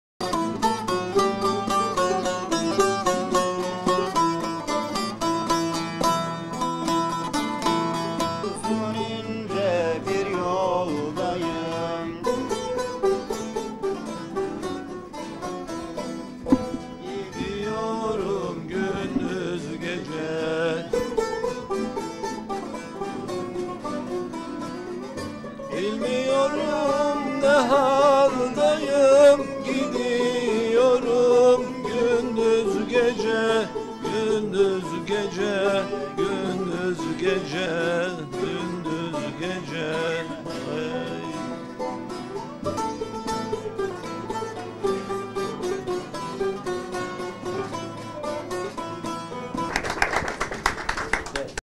Bakan Kasapoğlu türkü söyledi
Gençlik ve Spor Bakanı Mehmet Muharrem Kasapoğlu, Manisa'nın Turgutlu ilçesindeki gençlik merkezinin müzik atölyesinde öğrencilerle türkü söyledi.